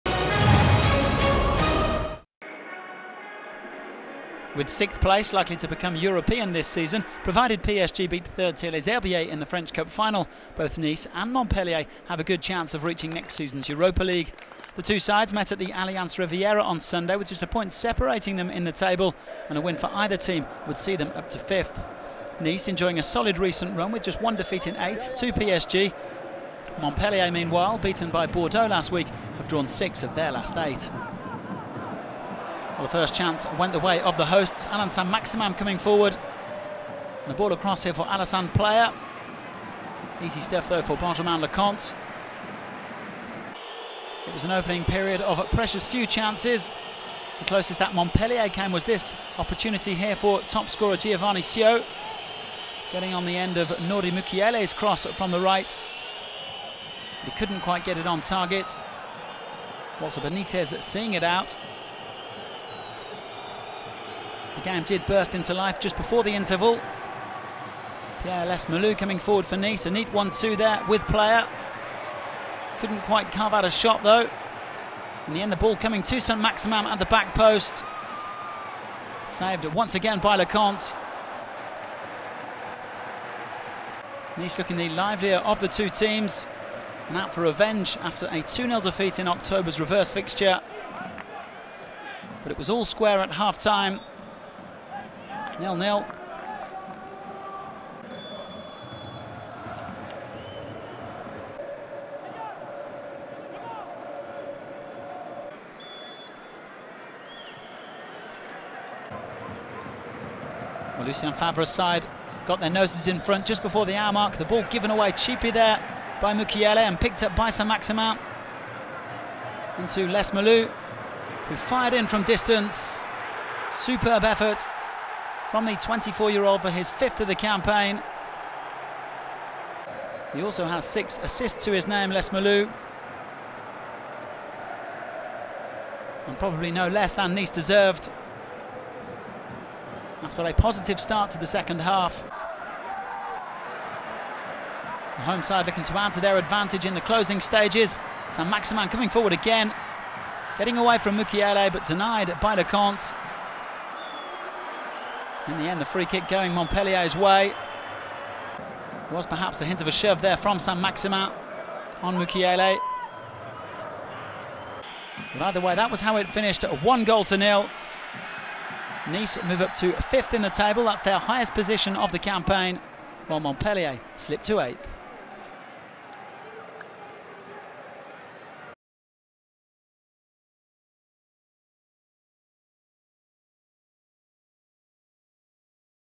OGC Nice - Montpellier Hérault SC ( 1-0 ) - Highlights - (OGCN - MHSC) / 2017-18
OGC Nice vs Montpellier Hérault SCHighlights ( 1-0) in video.

Ligue 1 Conforama - Season 2017/2018 - Week 34Allianz Riviera - Sun, 22 Apr 2018

Goals : Pierre LEES-MELOU (59' - OGC Nice)

OGC Nice Team line-up : 2 - Arnaud SOUQUET / 4 - Santos MARLON / 5 - Adrien TAMEZE / 6 - Jean Michael SERI / 7 - Allan SAINT-MAXIMIN / 8 - Pierre LEES-MELOU / 9 - Mario BALOTELLI / 14 - Alassane PLEA / 20 - Maxime LE MARCHAND / 31 - DANTE (c) / 40 - Walter BENITEZ